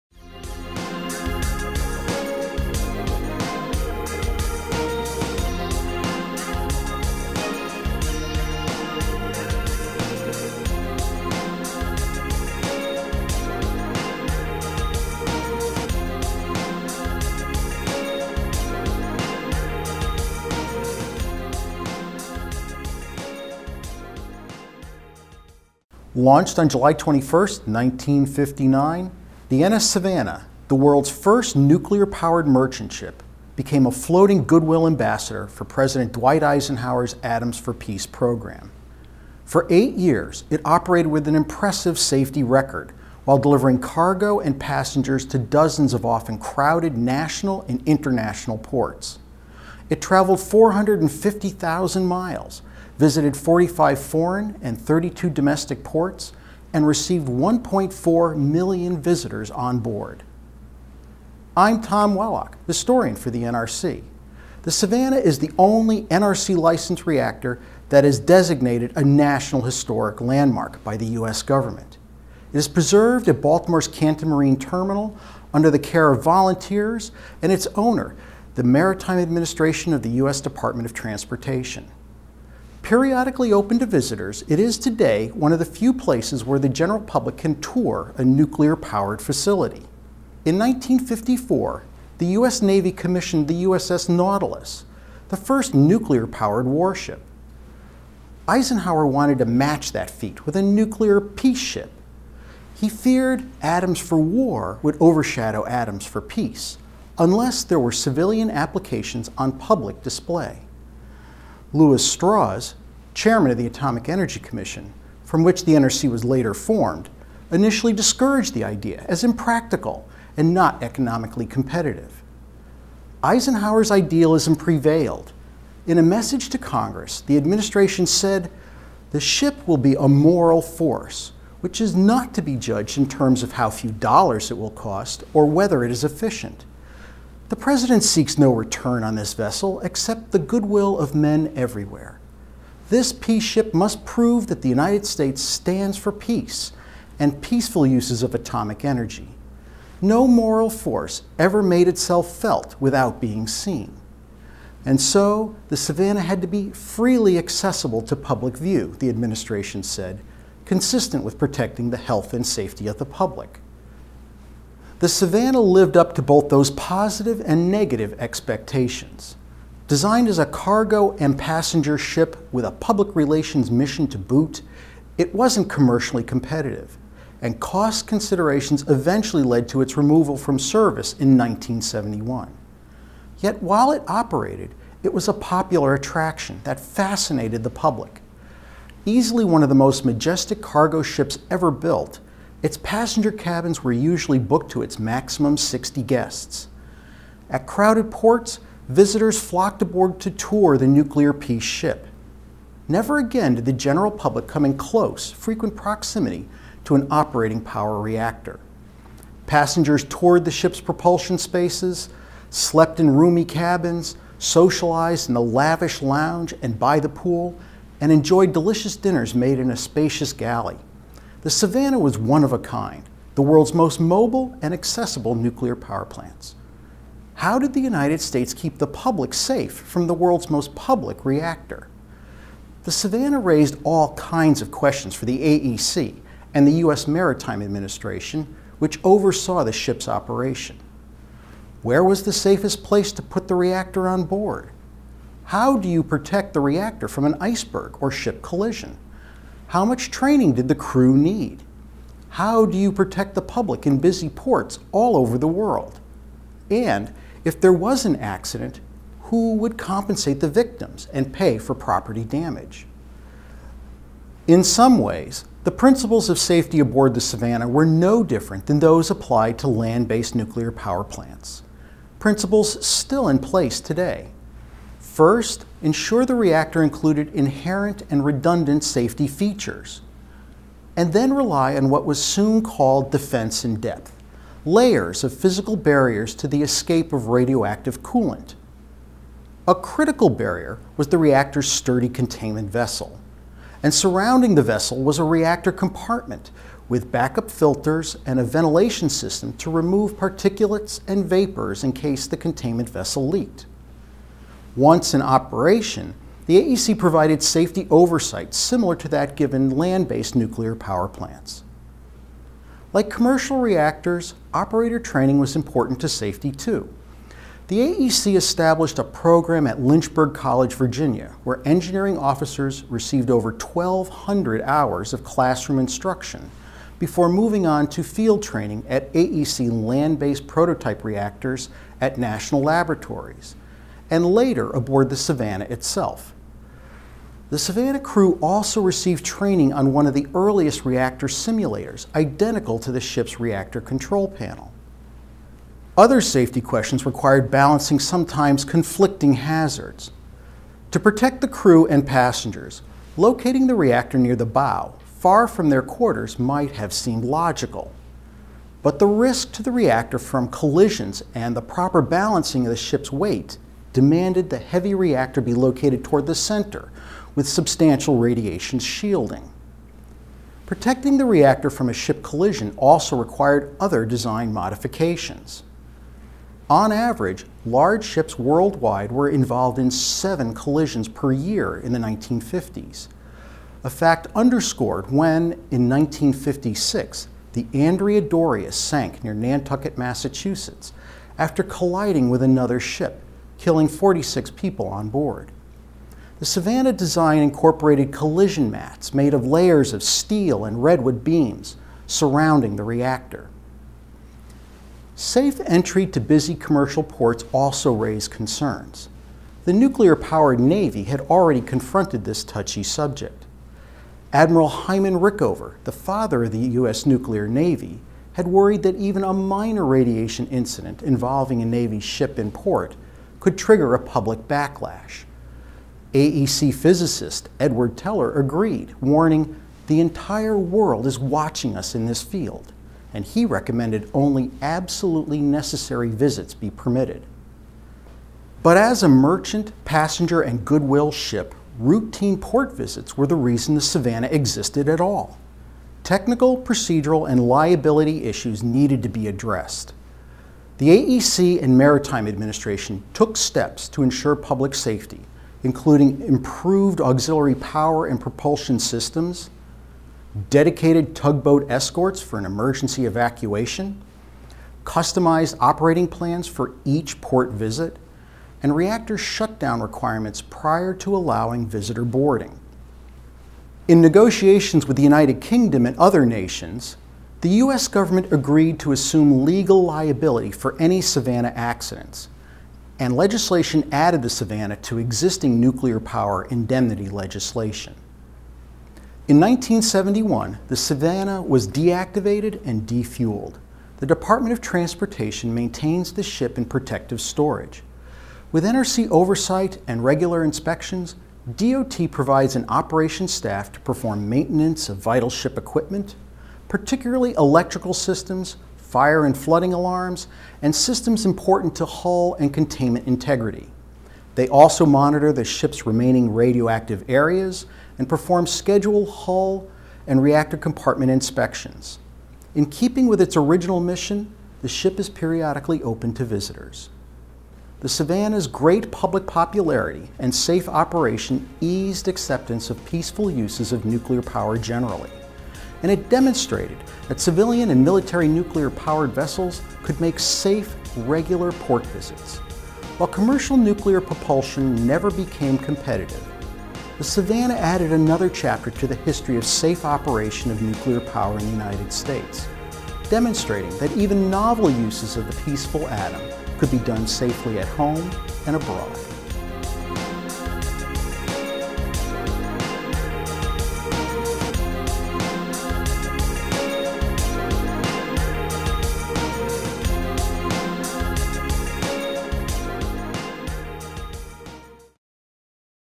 Moments in NRC History: NS Savannah Nuclear-Powered Merchant Ship -- July 21, 1959
This video is part of the Moments in NRC History Series. Watch
Watch Historian Tom Wellock in our new Moments in NRC History video talk about the only NRC-licensed reactor that is designated a National Historic Landmark by the U.S. government.  Learn how for eight years it operated with an impressive safety record while delivering cargo and passengers to dozens of often crowded national and international ports. It traveled 450,000 miles, visited 45 foreign and 32 domestic ports, and received 1.4 million visitors on board. The NS Savannah is preserved at Baltimore’s Canton Marine Terminal under the care of volunteers and its owner, the Maritime Administration of the U.S. Department of Transportation. Periodically open to visitors, it is today one of the few places where the general public can tour a nuclear powered facility.